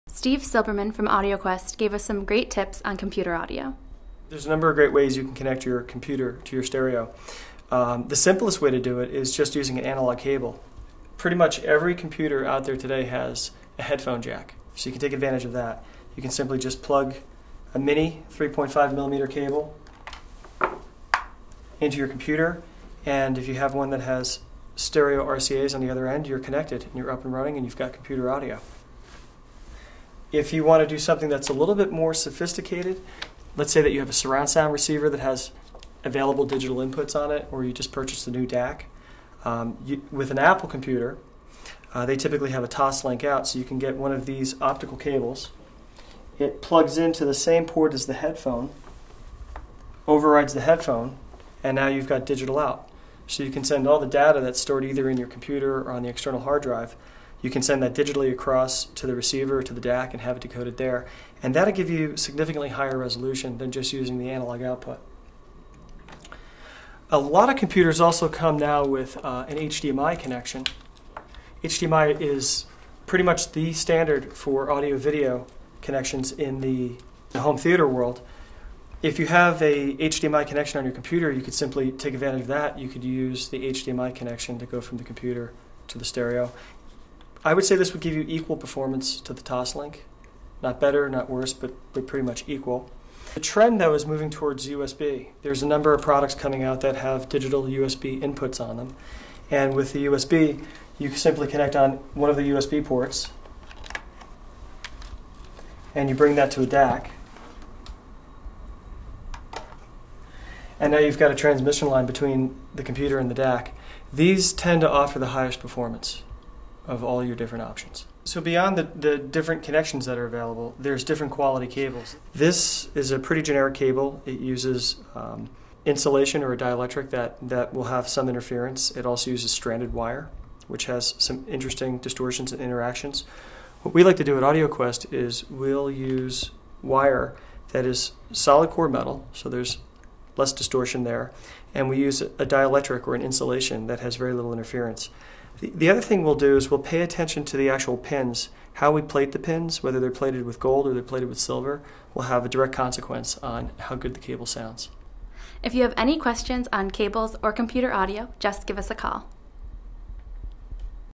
How to Connect Your Computer to Your Stereo | Crutchfield Video
Steve Silberman from AudioQuest gives us some tips for getting great audio from our computers.